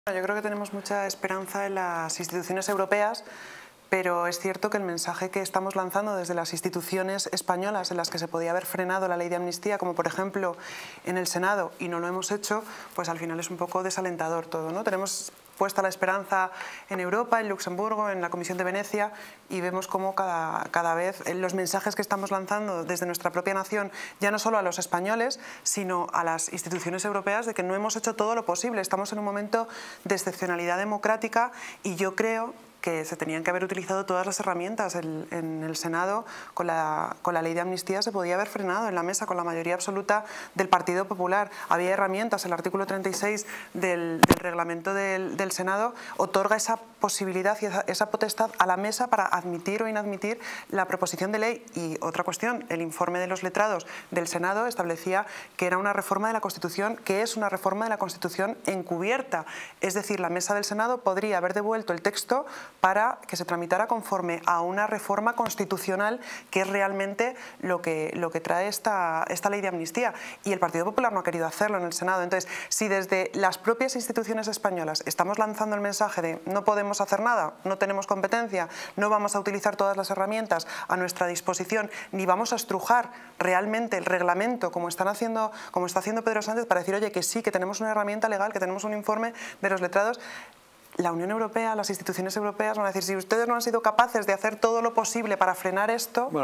🔴ISABEL PÉREZ, la LEY DE AMNISTÍA SE TENÍA QUE HABER FRENADO EN EL SENADO NO ESPERAR A EUROPA🔴
En esta entrevista, Isabel Pérez analiza la Ley de Amnistía y por qué cree que debería haberse detenido en el Senado en lugar de esperar a Europa. Míralo para saber más sobre este polémico tema en Distrito TV.